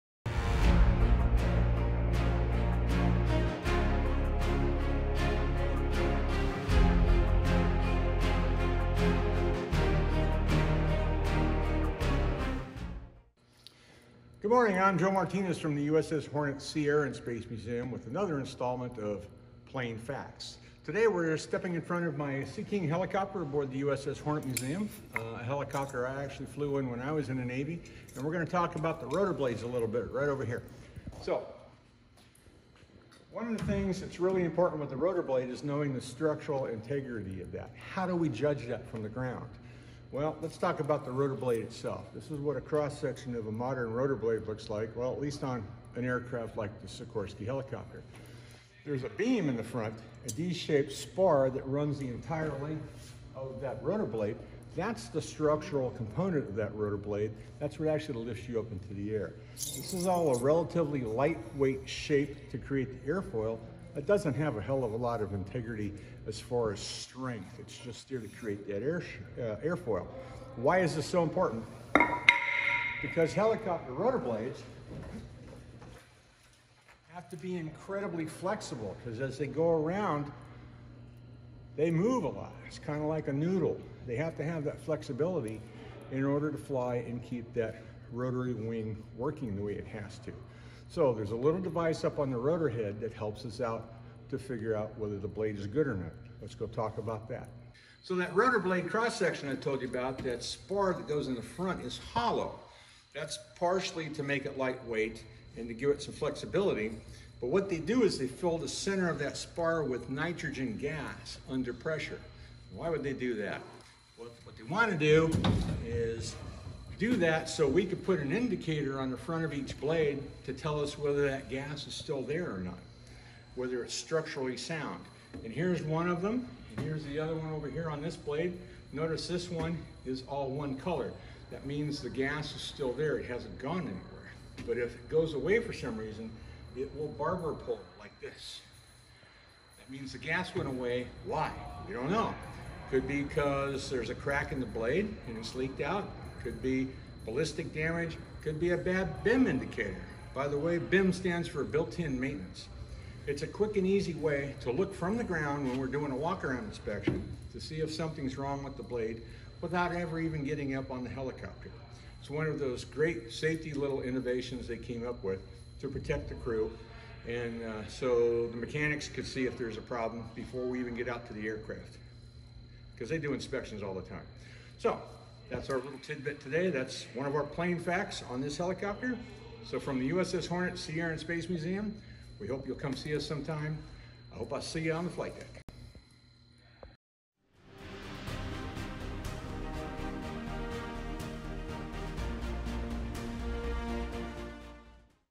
How are a Helicopter's Rotor Blades Maintained? | Plane Facts #3
In this episode, Joe shows us how ground crews examine helicopter rotor blades and how a BIM indicator determines is a blade needs to be replaced.